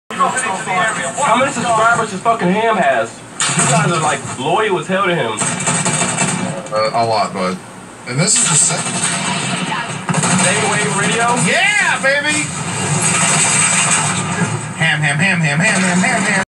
Daywave Radio Changing Lives For The Better
This guy was so amazed when daywave radio raied his live gaming stream. he was a good sport and he even said he couldn't wait until he told his mom about what happened. you never had more than 30 likes on a video and we got him up over 120.